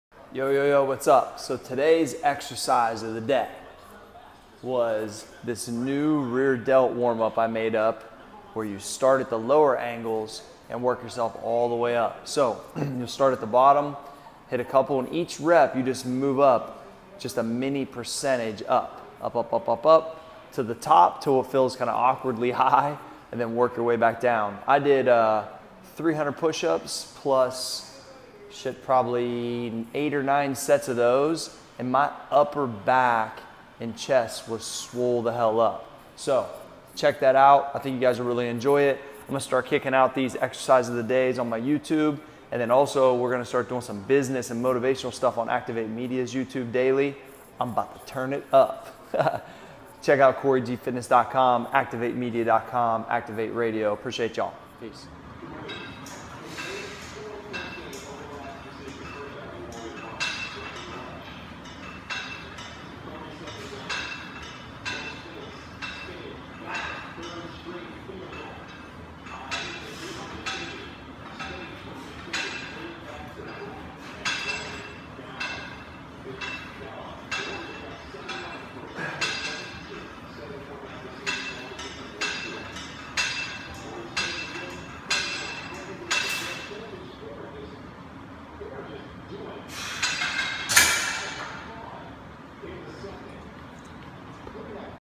Around the World Cable Rear Delt Flyes
Here is an exercise I have done in 1 or 2 of these angles but not all of them. I did 8 sets of going up & down & it had my back super swole up

Corygfitness.com